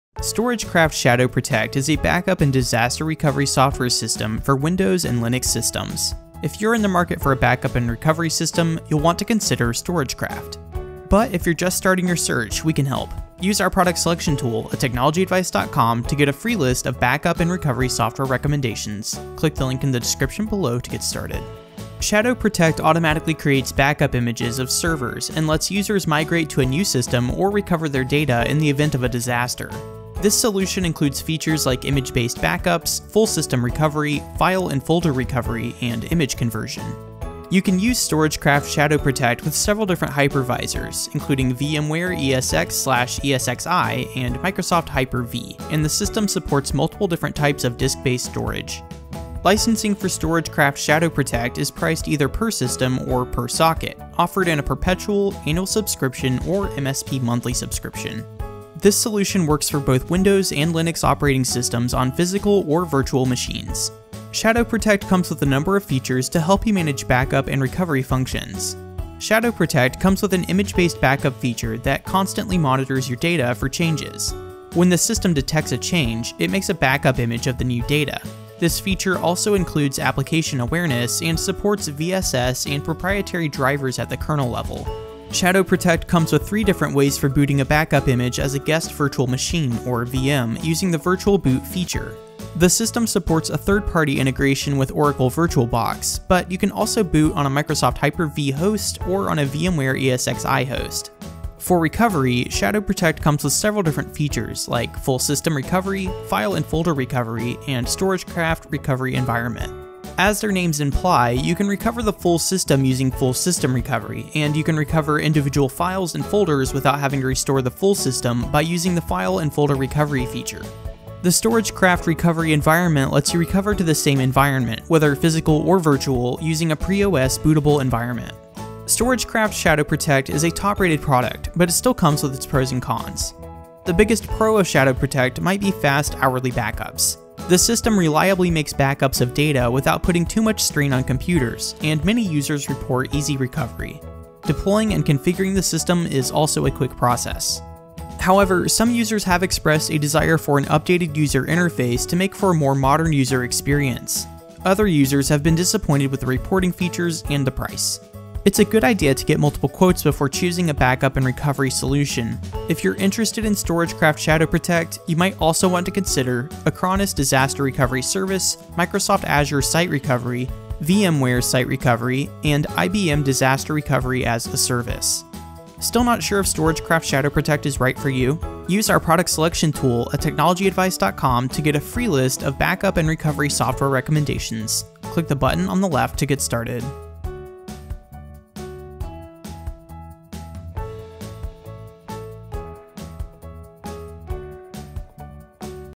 StorageCraft ShadowProtect Review
This video offers a review of StorageCraft ShadowProtect. Looking for a backup and recovery solution? Get a list of free, personalized recommendations using our Product Selection Tool

➤ CHAPTERS
0:00 Intro
0:21 What is StorageCraft ShadowProtect?
1:04 Key features
2:08 Pros and cons
2:38 Alternatives
2:56 Get free backup and recovery recommendations!